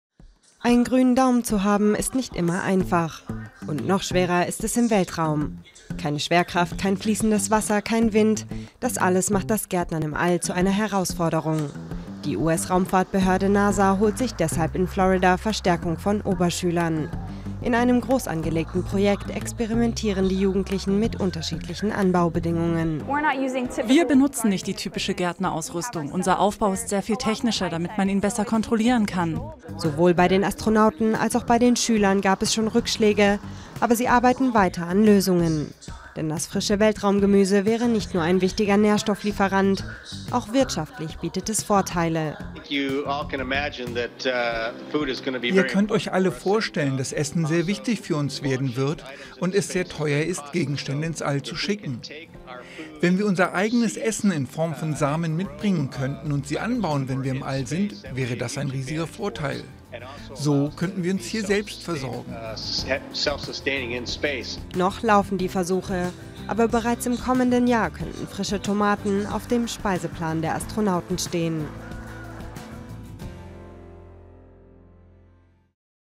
Grüner Daumen im All? Nasa forscht an Weltraumgemüse
Einen grünen Daumen zu haben ist nicht immer einfach. Und noch schwerer ist es im Weltraum. Die US-Raumfahrtbehörde Nasa holt sich in Florida deshalb Verstärkung von Oberschülern, die ihnen neue Ideen geben sollen. In einem großangelegten Projekt experimentieren die Jugendlichen mit unterschiedlichen Anbaubedingungen.